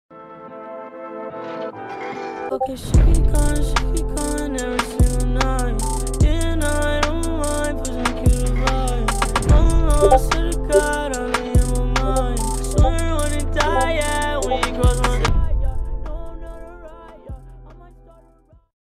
「HOPE 🤍」「AMV/EDIT」(sumioc8)
This is my first neptune style i hope you guys like it (This Is A Open Collab) So the second part is what i made

0:00 sumi.0n
0:09 ChicFlecMan